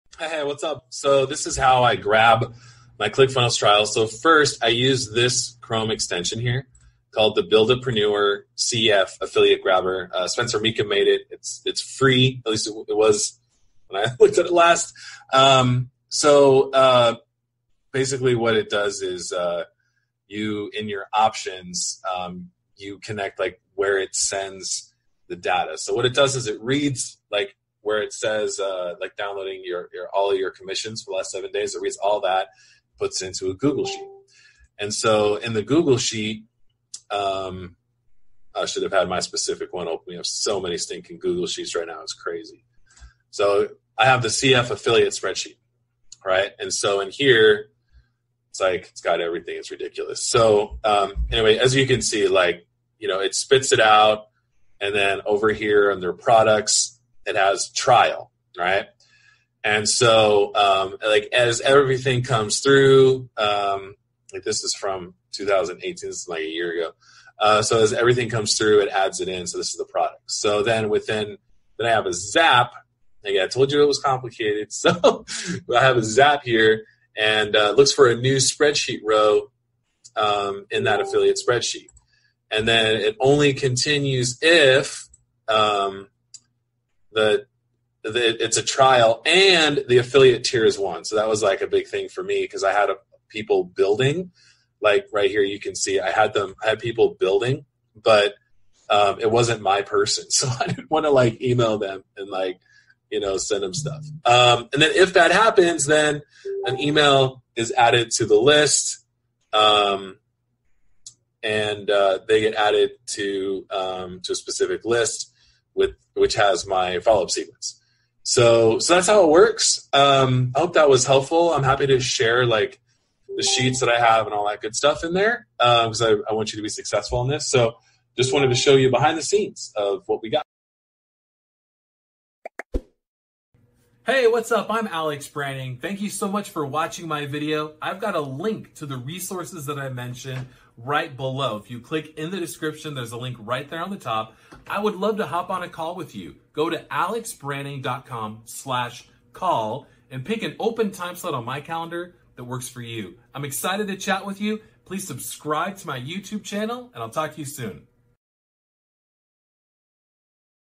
ClickFunnels Affiliate Tips: Set-Up Automation so New Trials Get Bonuses Right Away
In this short video Alex Branning shows how to track new ClickFunnels trials and get them set up with your email follow-up sequence...

This process uses the following tools: Buildapreneur's Chrome Extension, Google Sheets, and Zapier